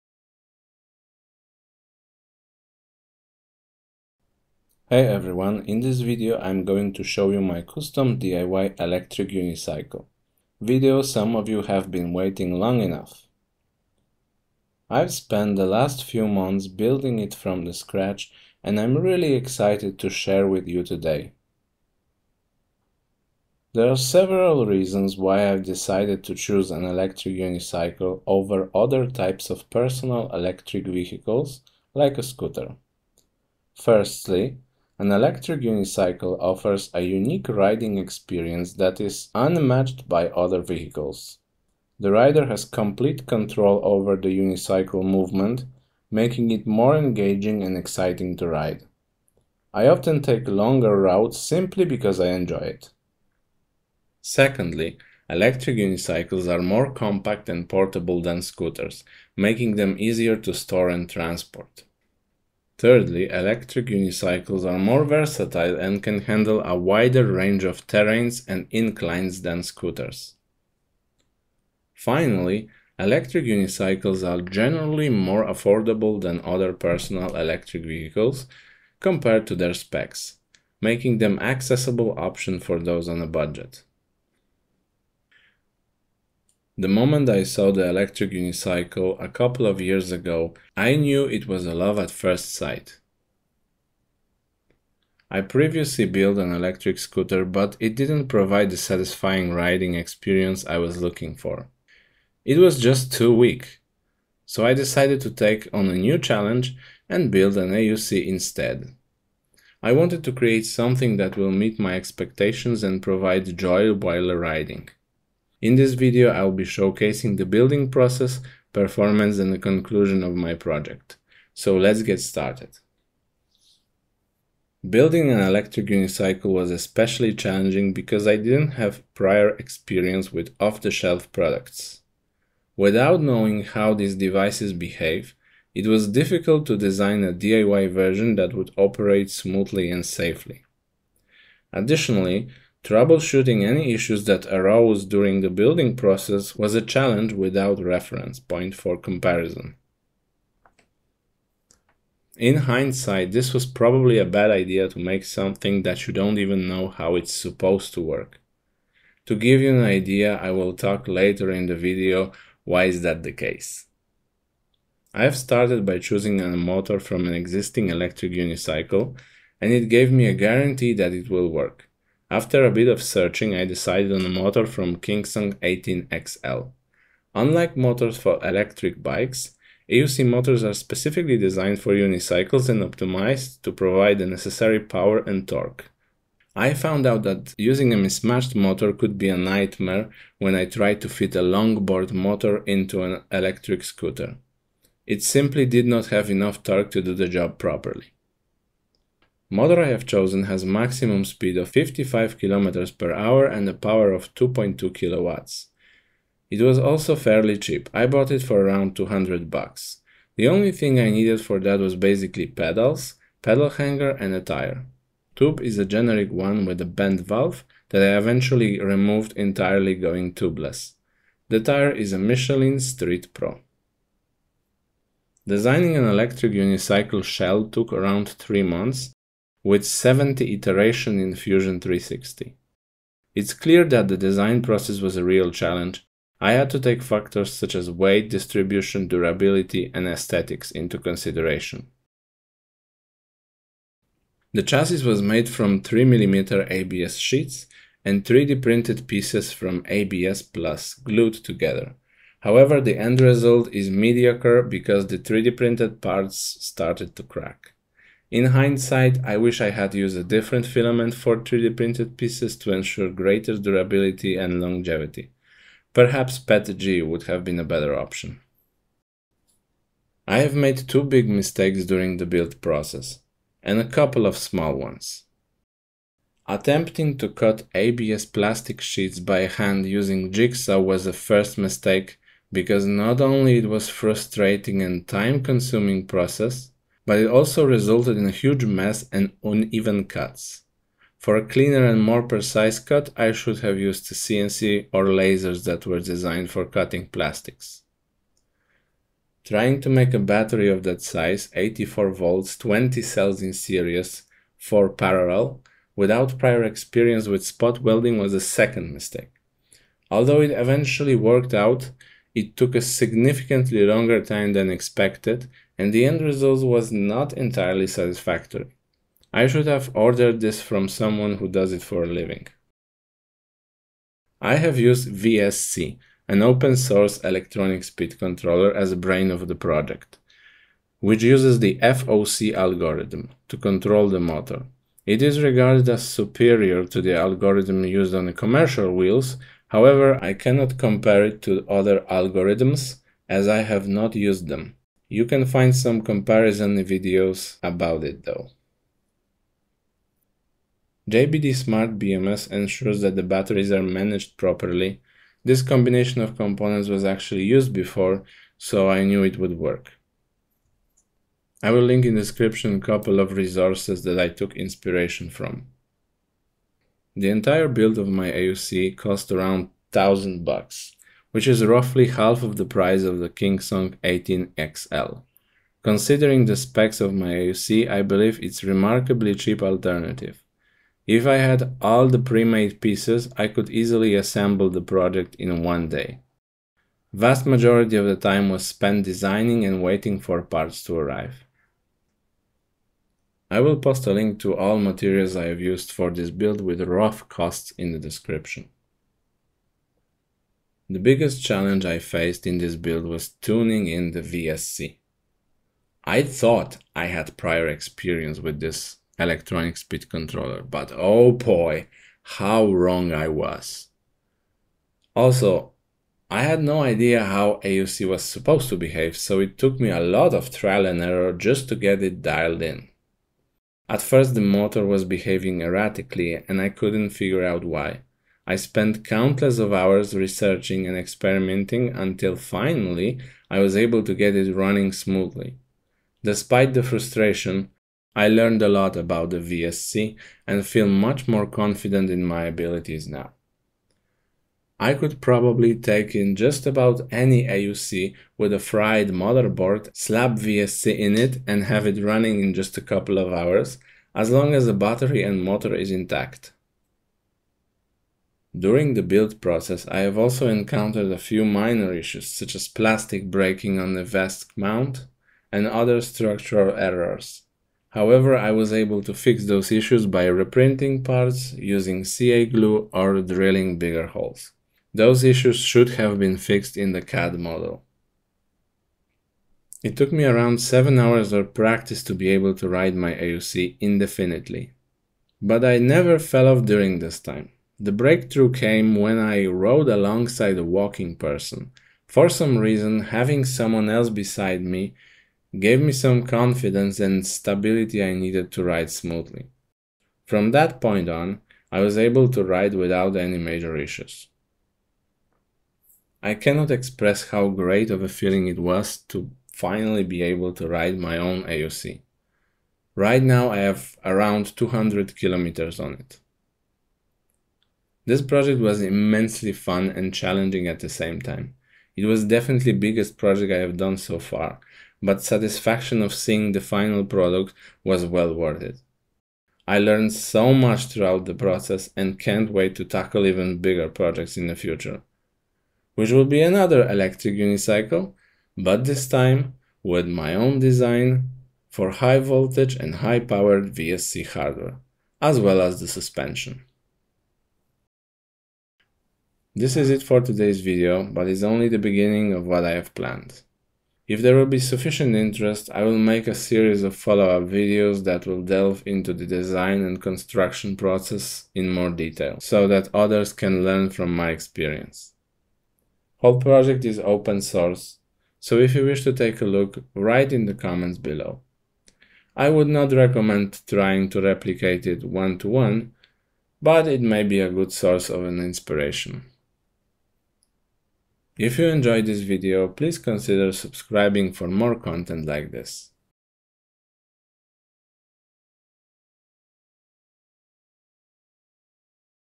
DIY Electric Unicycle (EUC) UniVesc - Details
How to make custom diy electric unicycle using vesc electronic speed controller, long version with details and 3D CAD model

Special thanks to all folks at EUC and VESC discord for responding to insane amount of questions and helping me to learn how to ride.